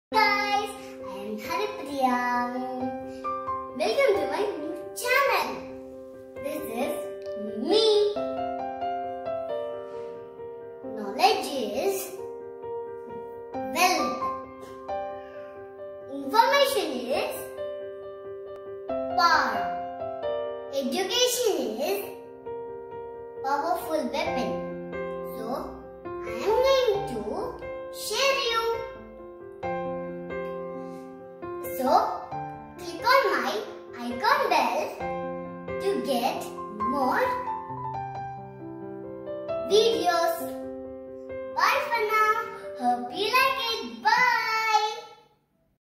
Welcome to my channel!!
Haripriya,5 year old started new YouTube channel "This is ME" to share her knowledge and information.